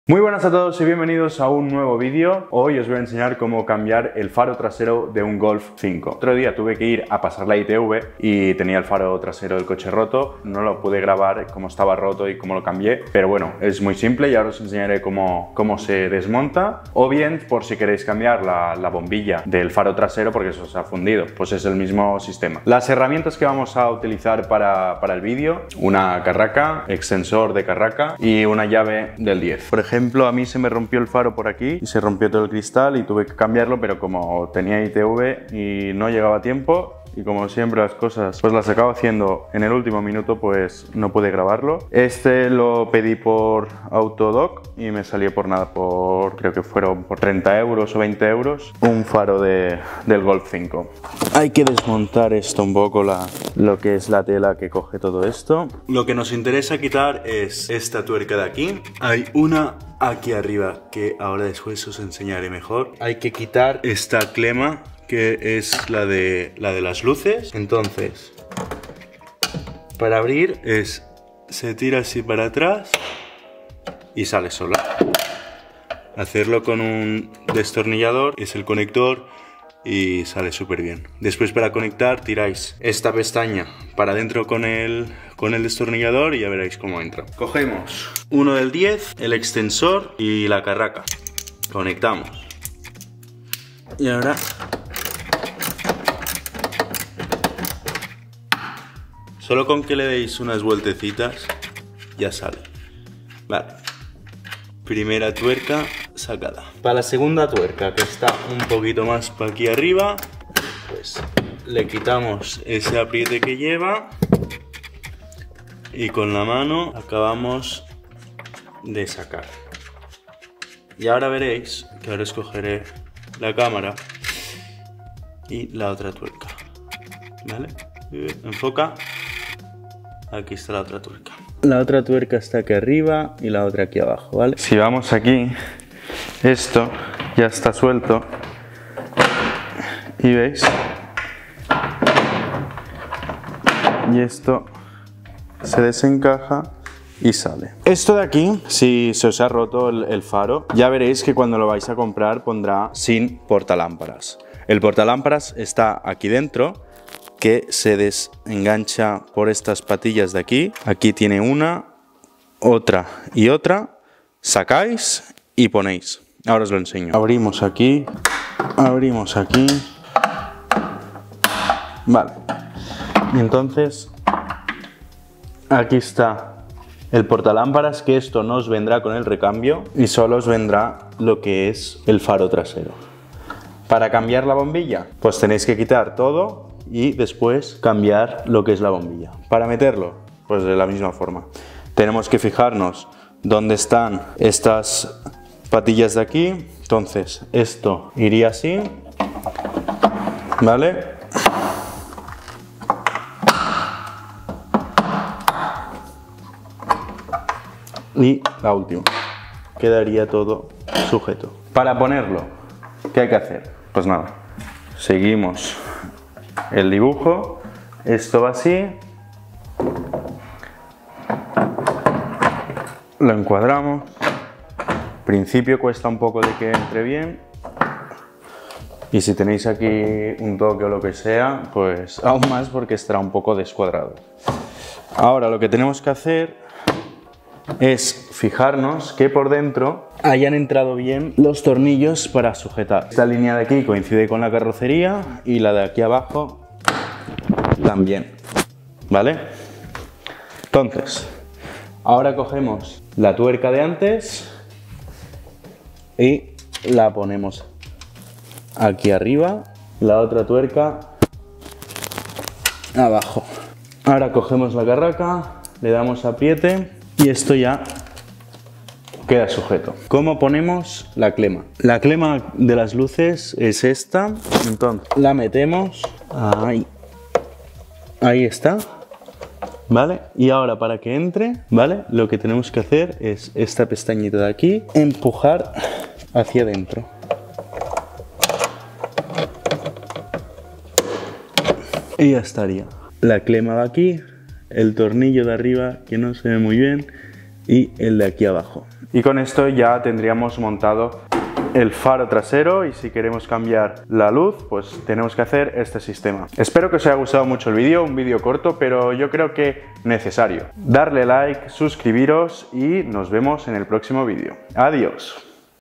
CÓMO CAMBIAR FARO TRASERO GOLF 5
¡HE VUELTO! Os traigo un video corto de cómo cambiar el faro trasero de un golf 5, por si no sabéis cómo hay que hacerlo si queréis cambiar las luces o bien porque se os ha roto el faro por un golpe, en este video podréis ver cómo se hace.